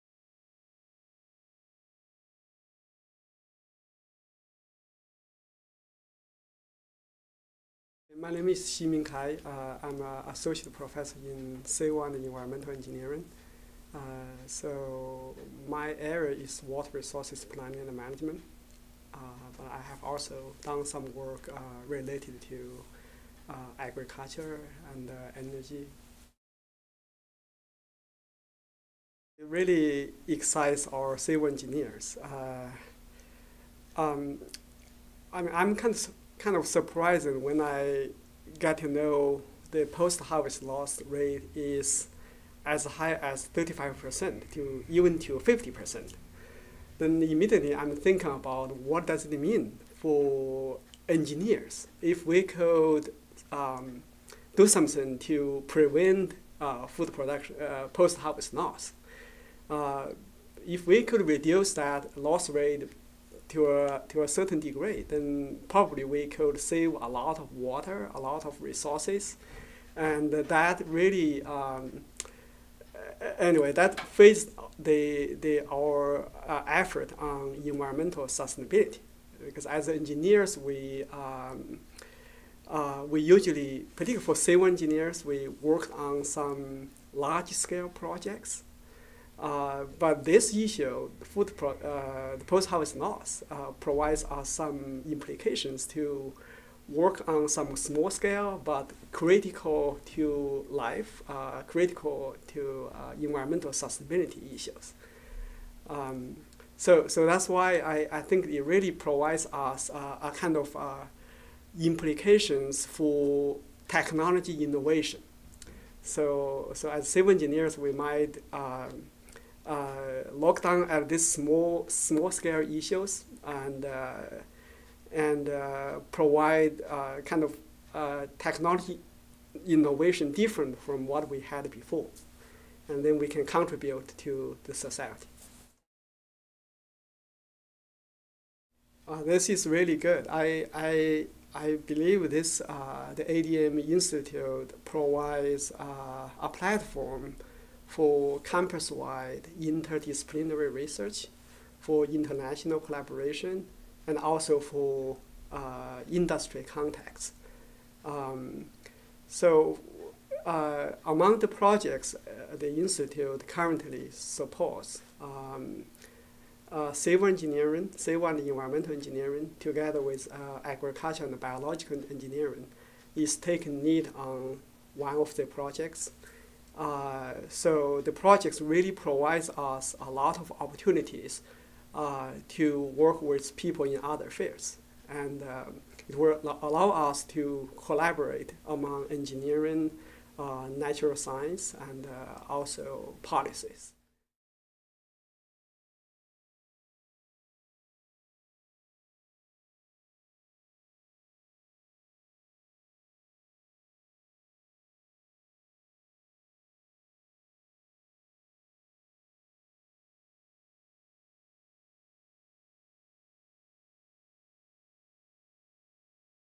ADM Institute Researcher Interviews - Ximing Cai
In 2012, the ADM Institute for the Prevention of Postharvest Loss invested $2.1 million in seven research projects focused on efforts to reduce post-harvest loss. The institute has prepared short interview videos with one of the lead researchers for each of these projects identifying their views regarding why research should be conducted in this area, current challenges, and what success of their projects would look like.

Dr. Ximing Cai discusses the relevance of post-harvest loss to civil engineers, particularly in regard to environmental sustainability, technological innovation, and conserving resources. He also highlights the collaborative opportunities made available by the ADM Institute for the Prevention of Postharvest Loss at the University of Illinois at Urbana-Champaign.

To learn more about this specific research project, as well as other related activities, please visit the ADM Institute for the Prevention of Postharvest Loss's